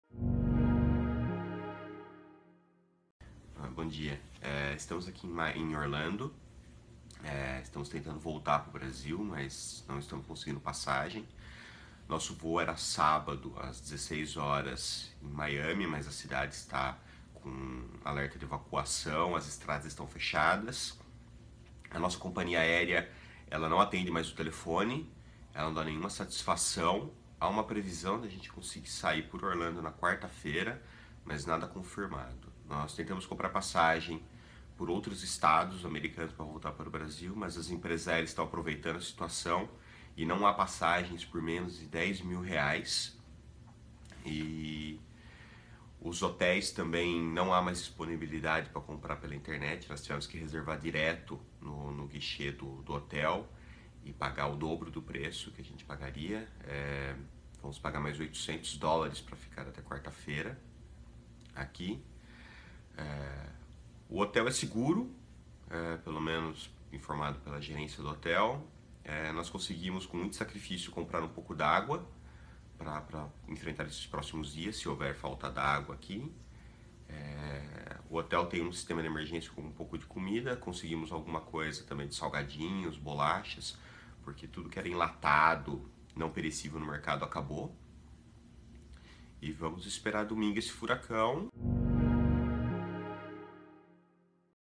Pai pede ajuda para fugir de furacão na Flórida
Família 'presa' na Flórida relata cancelamento de voo e passagens por mais de R$ 10 mil.
Grupo de Jundiaí (SP) está em Orlando e deveria retornar ao Brasil no sábado (9), mas voos foram cancelados. Outra família desistiu da viagem por conta do furacão Irma.
ma família de Jundiaí (SP) que está em Orlando, nos Estados Unidos, há quase 15 dias, enfrenta dificuldades para voltar ao Brasil por causa do furacão Irma, que deixou 14 mortos até a manhã desta sexta-feira (8). Ao G1, o gerente de projetos Adriano Ricardo Cappatto contou que as passagens chegam a custar mais de R$ 10 mil.

O G1 acompanha em tempo real o trajeto do furacão Irma

Adriano diz que a esposa, Simone Capatto, viajou a trabalho para Miami em agosto e, dias depois, ele foi para lá com o filho de 7 anos. A família, então, foi passear em Orlando e deveria retornar ao Brasil neste sábado (9).

No entanto, o voo de volta para casa - que seria por Miami - foi cancelado. Agora, a família está tendo dificuldades ao tentar remarcá-lo. A previsão é que consigam retornar apenas na quarta-feira (13) e, para isso, a família terá que desembolsar US$ 800 para os dias extras no hotel.

    "Nossa companhia aérea não atende mais o telefone, não dá mais nenhuma satisfação. Tentamos comprar passagem por outros estados americanos para voltar ao Brasil, mas as companhias estão aproveitando a situação e não há passagens por menos de R$ 10 mil", afirma Adriano.

O G1 questionou a companhia aérea Avianca sobre a dificuldade do consumidor em contatar a empresa, mas ainda não recebeu um posicionamento.

O gerente de projetos também relatou ao G1 a dificuldade na compra de água e a escassez de produtos nos supermercados (veja o vídeo abaixo).
Malas prontas

Enquanto a família de Adriano e Simone tenta voltar ao Brasil, outra família de Jundiaí adiou a viagem para os Estados Unidos um dia antes do embarque, também por causa do furacão Irma. Ligia Sposito, de 38 anos, embarcaria nesta sexta (8) com o filho, de 6 anos, e o marido para Miami.

A viagem, marcada desde fevereiro e que custou cerca de R$ 10 mil, ainda incluía um cruzeiro nas Bahamas. No entanto, o rastro de destruição do furacão assustou a publicitária e ela decidiu de última hora não viajar mais.